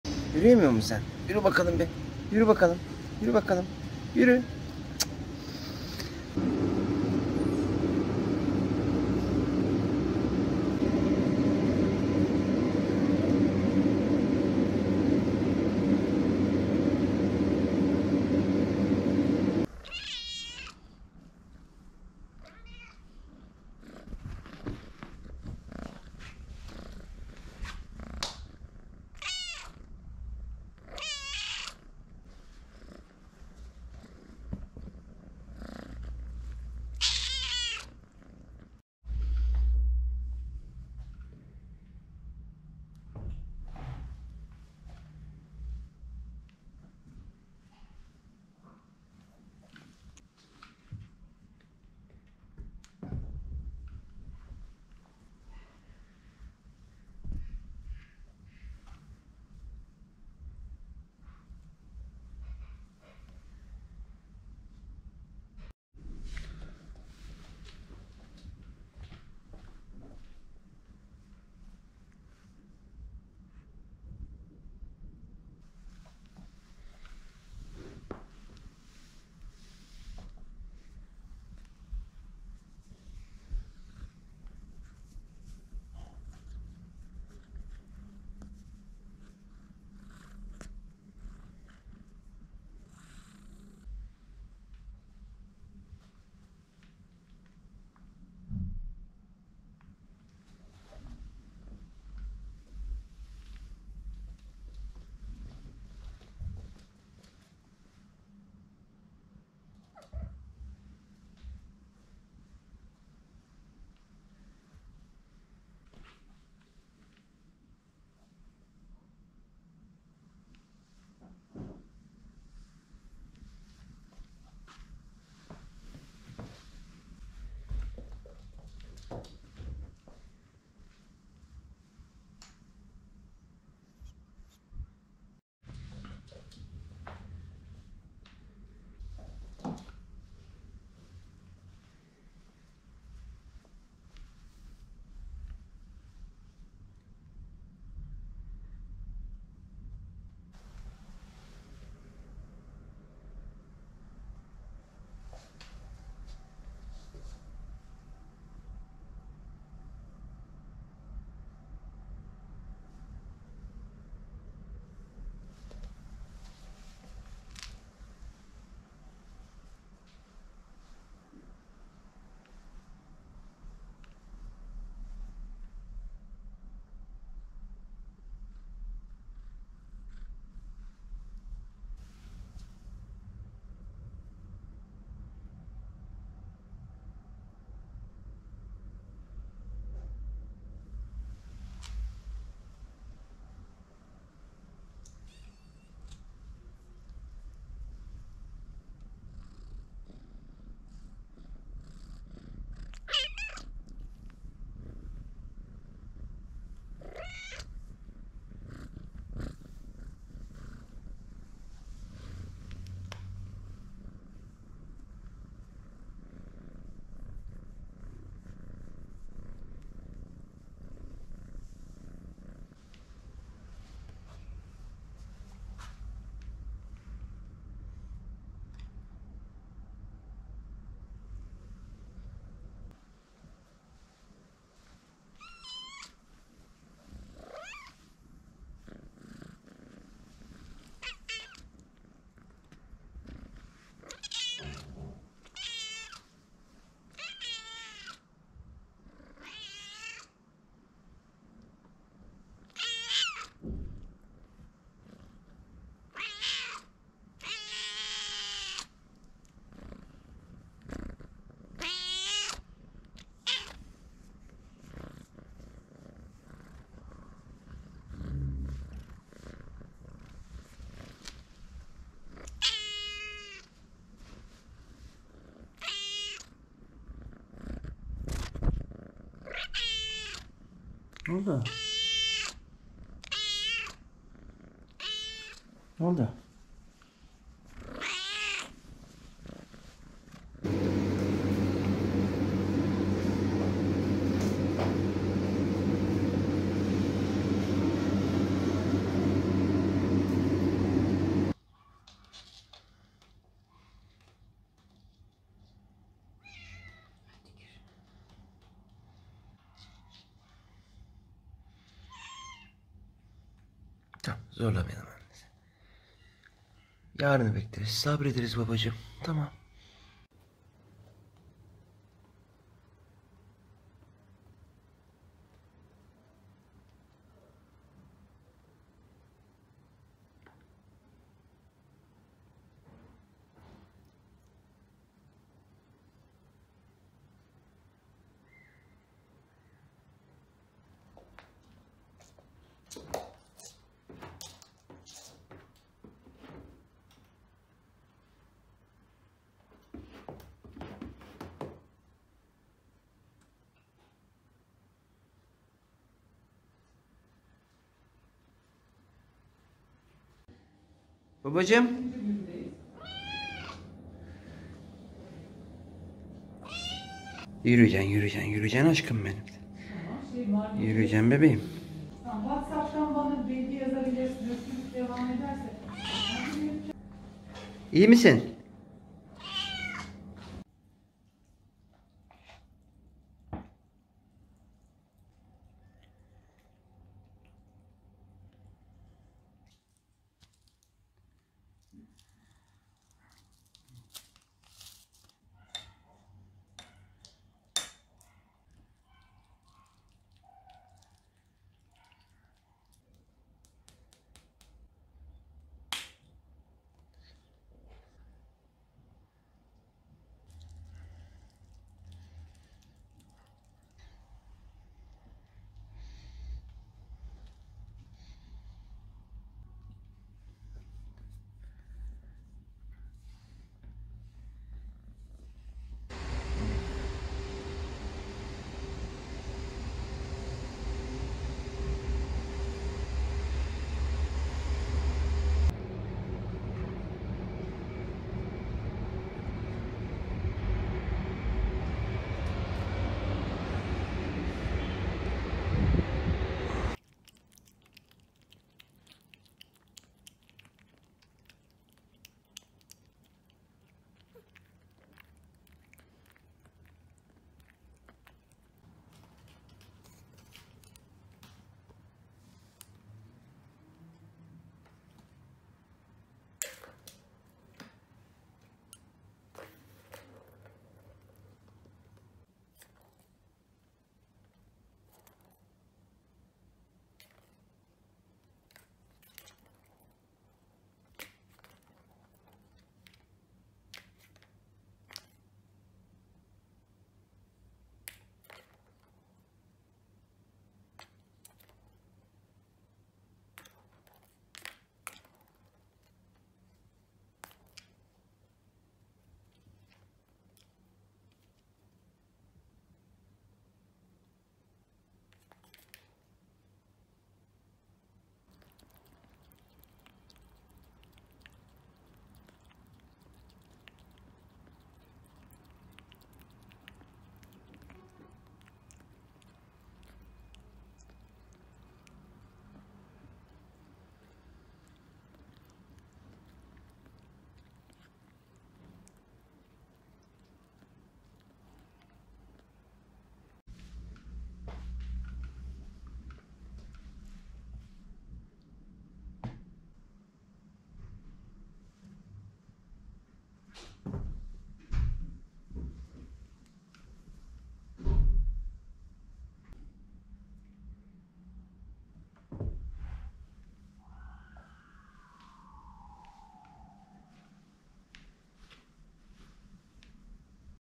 The paralyzed cat that could not walk a month ago is now very healthy.
The paralyzed kitten, who could not walk, started to fly after treatment.
We are grateful to the veterinary clinic, which has made outstanding efforts to treat Asia. Since video shooting is prohibited in the veterinary clinic, we will tell you about the treatment process in our home.

*Treatment in the veterinary clinic for 17 days. (Many vaccines. Many vitamin supplements)
*Vitamin support: Along with yoghurt. calcium, vitamin d3, phosphorus, magnesium, vitamin k2
*Collagen support: One of the basic proteins involved in tendon, bone, muscle, connective tissue and skin is one of them.
*A warm environment. A warm bed.
*Excessive love and attention.
*Sunlight so that she gets the necessary vitamins.
*Regular daily sports.
*Drinking cow trotter soup.
*Feed her with boneless fish meat.

This kitten, born in a basement, has become unable to walk day by day.  Of course we will not leave her on the street. The partially paralyzed cat is now in our house. With a new home, quality cat food, daily treatment and love, it will be better than before. X-rays were taken. There is a congenital spinal developmental disorder. Corona and rickets related diseases. A long course of treatment awaits us. The nervous system has lost its function.